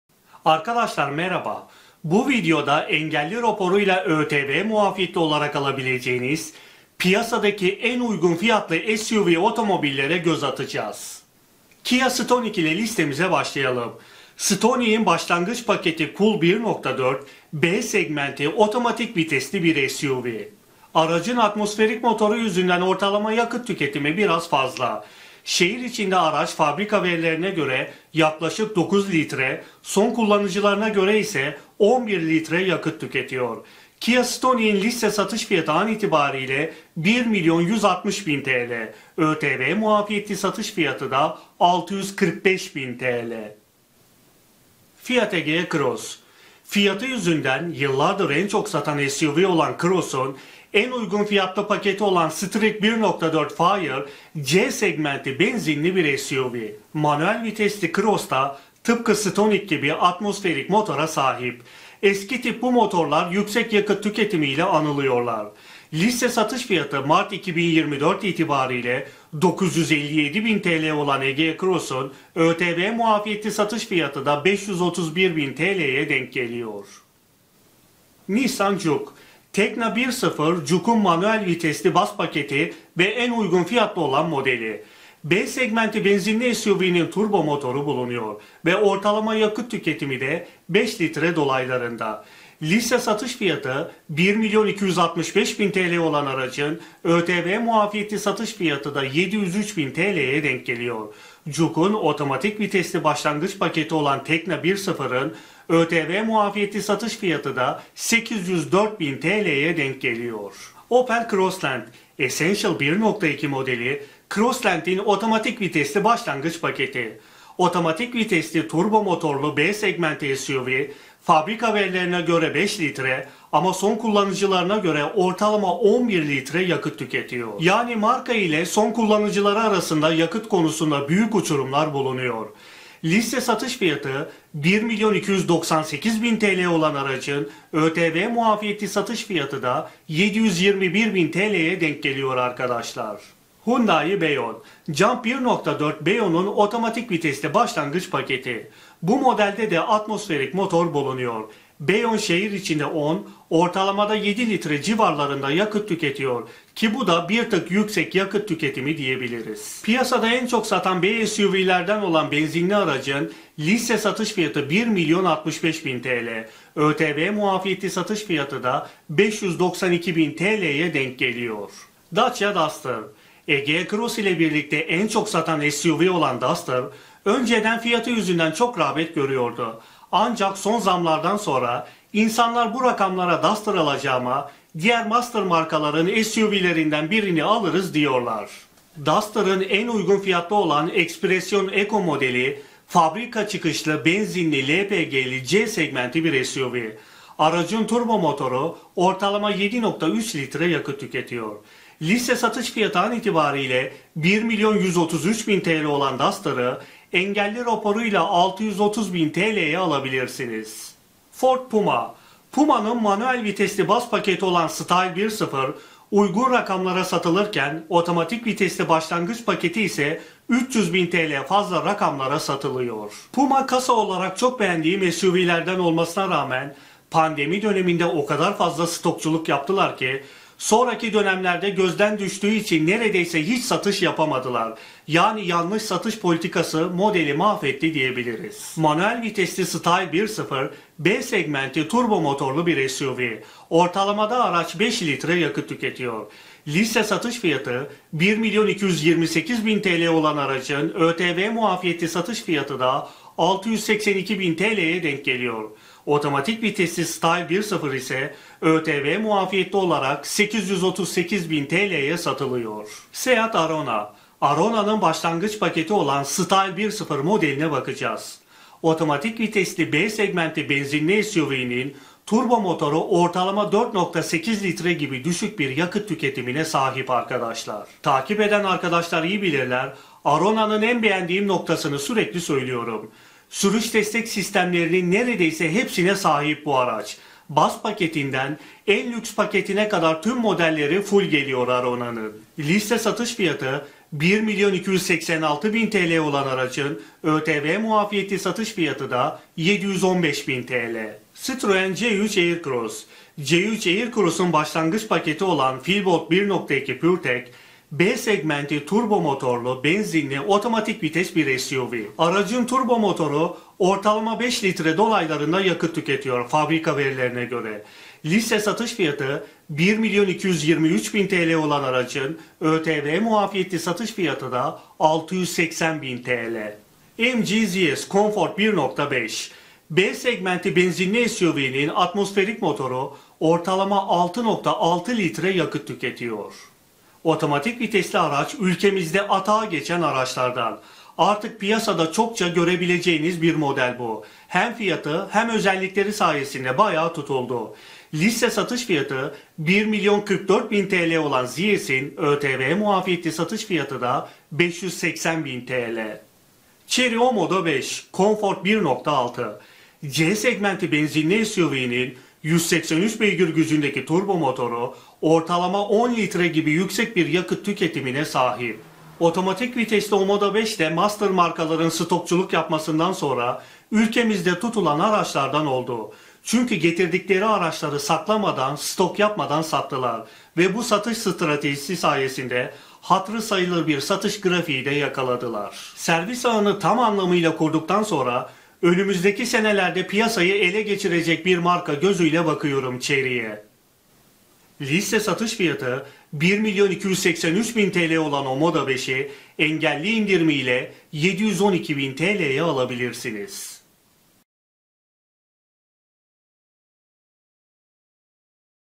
Engelli Raporuyla En Uygun Fiyatlı SÜPER SUV OTOMOBİLLER
Engelli raporuyla ötv indirimli olarak alabileceğiniz uygun fiyatlı ama kaliteli suv otomobillerin  fiyat incelemeleri...